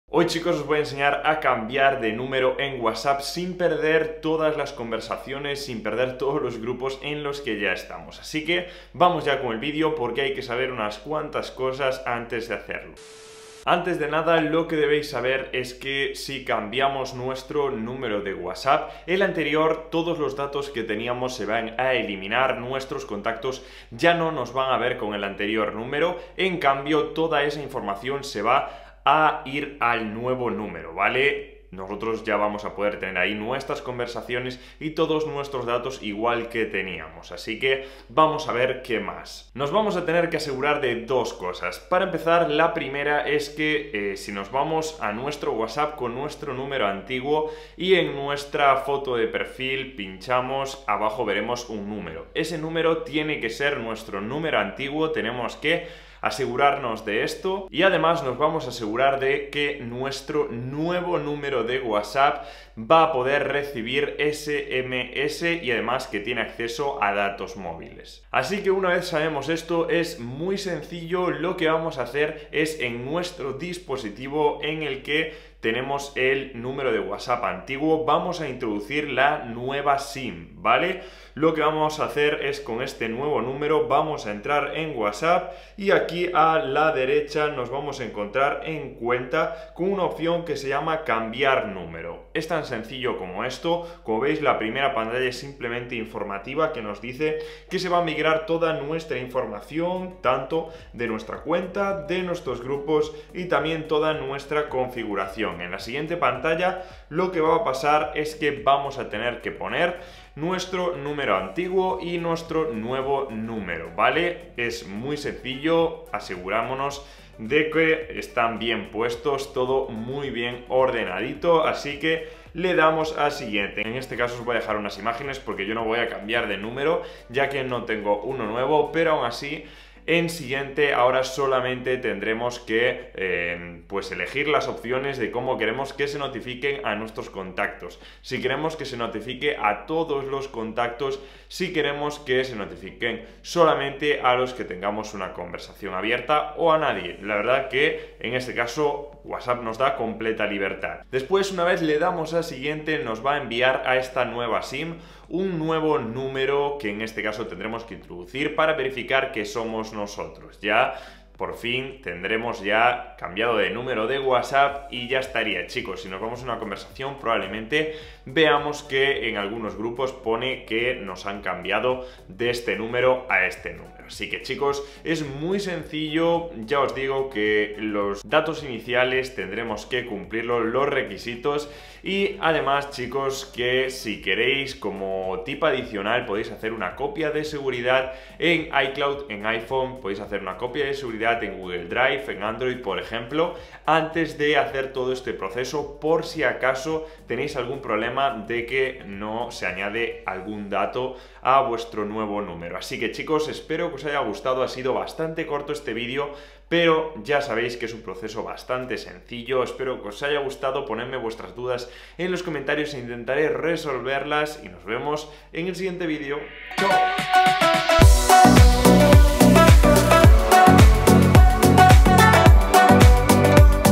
Como CAMBIAR DE NUMERO en Whatsapp sin perder conversaciones
Si has cambiado de número de móvil o compañía y no quieres perder ningún contacto, grupo ni conversación, hoy te enseño una forma muy fácil e integrada en la propia aplicación para traspasar todo en menos de un minuto.